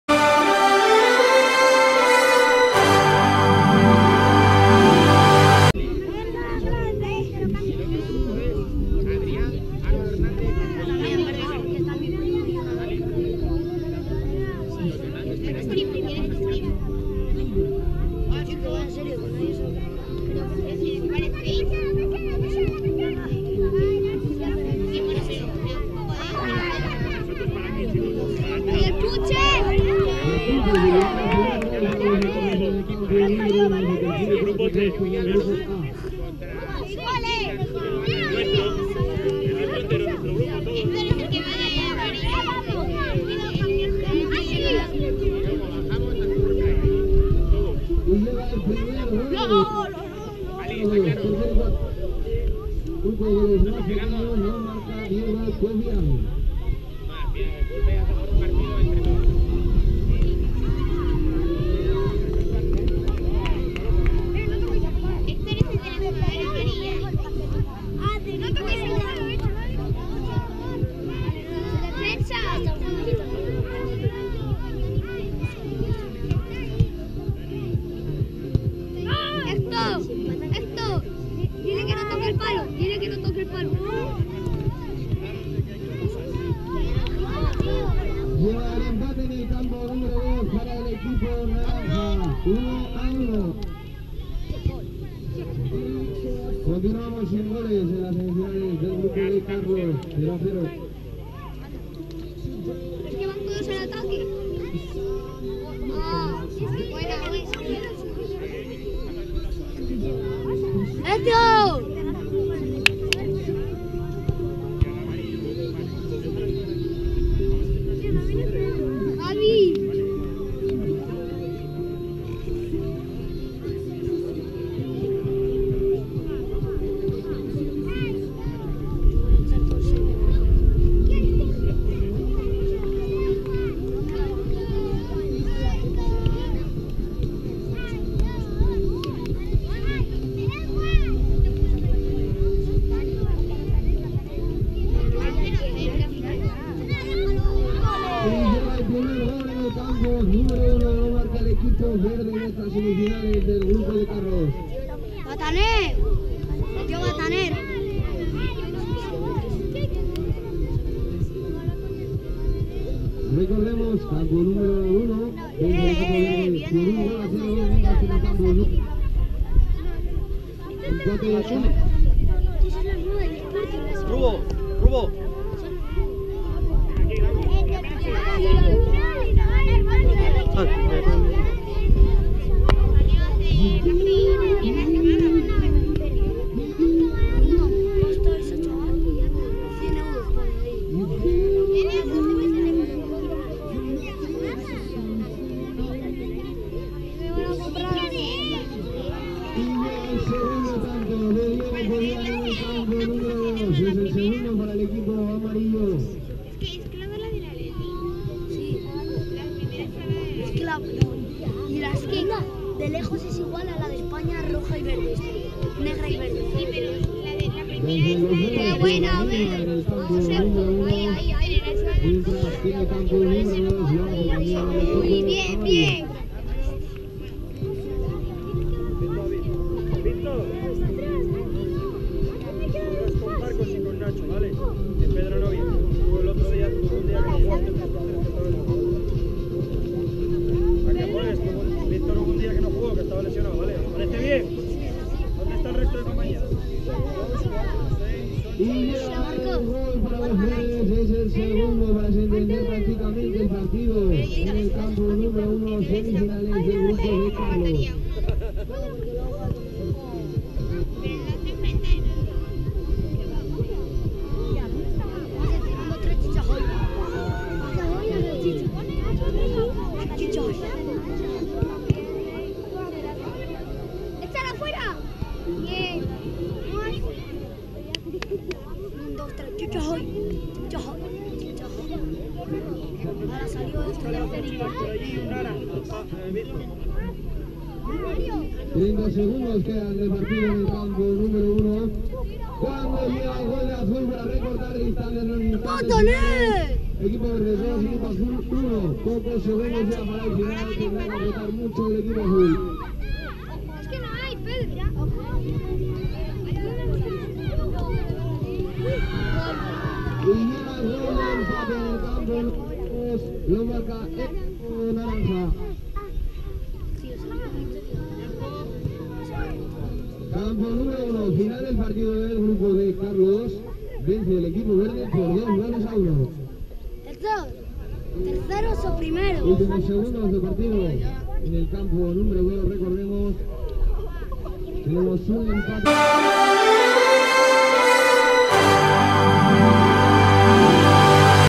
Campus16 T4 partido1 carlos
Finales de 3x3 del Campus de fútbol de Mareo 2016 (Turno 4)